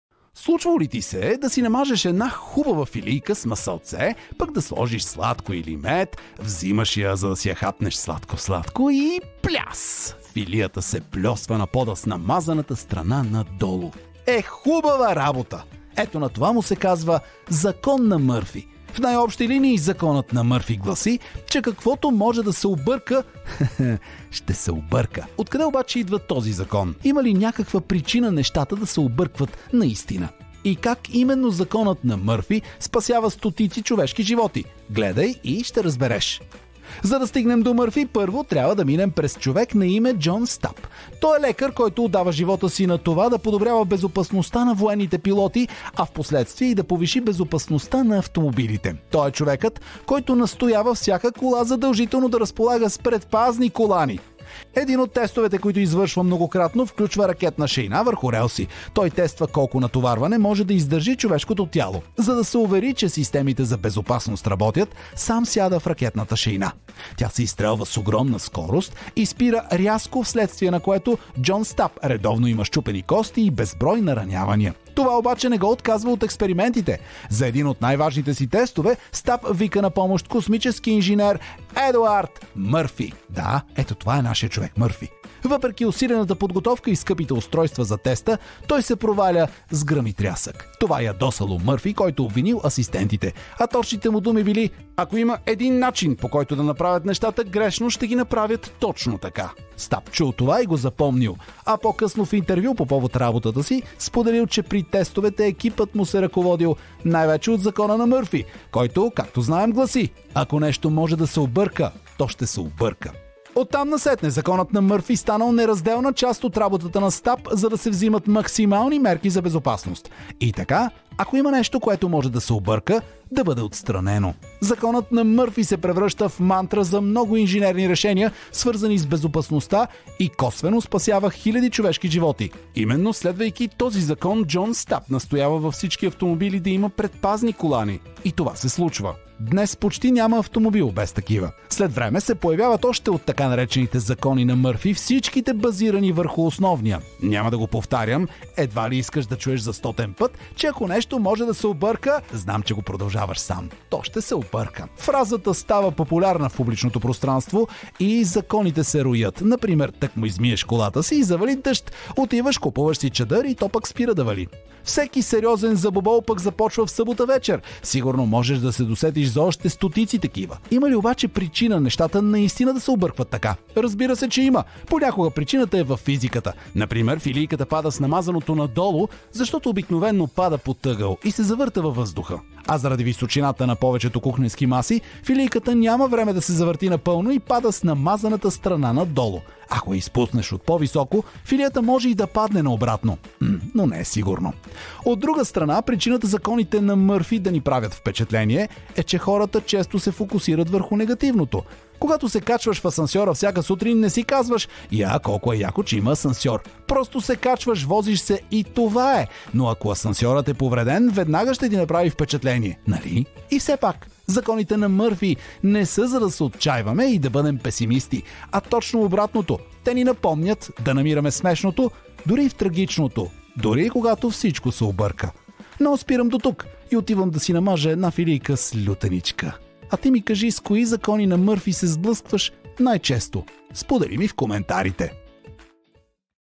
Как Законът на Мърфи спасява хиляди човешки животи
Най-вероятно си чувал за закона на Мърфи. Как обаче се е появил този закон и как спасява хиляди човешки животи? Изгледай видео урока докрай и ще разбереш!

Всичко започнало с лекаря Джон Стап и един негов тест, в който участие взел и космическият инженер Едуард Мърфи. Нещата обаче малко се объркали и така се зародил Законът на Мърфи, който е актуален и до днес.

Ела на и си научи за училище бързо и лесно с още над 21 000 видео урока, теста и преговора с мисловни карти!